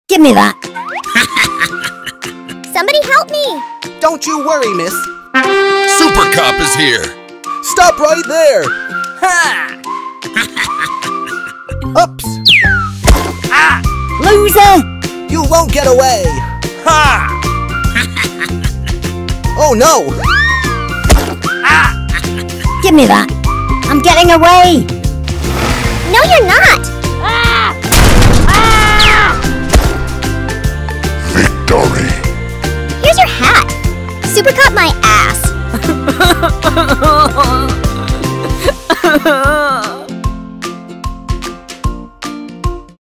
Vengeful Cat Takes Justice Into Her Own Hands
A thrilling adventure in ‘Vengeful Cat Takes Justice Into Her Own Hands’!
In this captivating tale, witness an unlikely hero—a vengeful cat—as she embarks on an epic quest to surprise a mischievous raccoon thief and recover a stolen purse. Booby traps, stolen hats, and unexpected twists await in this heartwarming animation that celebrates bravery and justice.